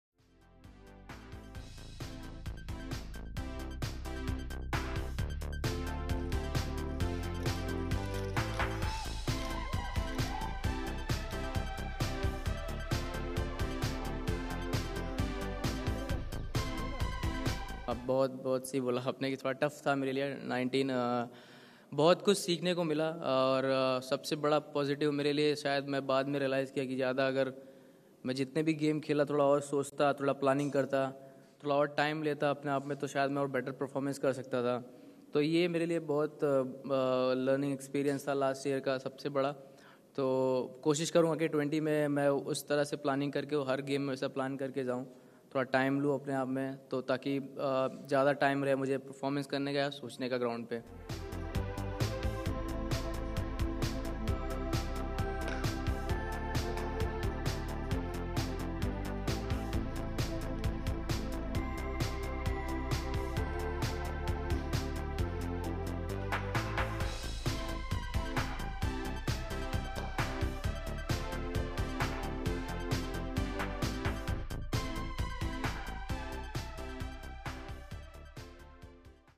Ind vs SL: Will try to plan every game better in 2020, says Kuldeep Yadav
Pune (Maharashtra), Jan 09 (ANI): Indian left-arm spinner Kuldeep Yadav admitted that 2019 was a tough year for him. “In 2020, I will try to plan every game better and give myself more time, so I get more time to reflect on my game.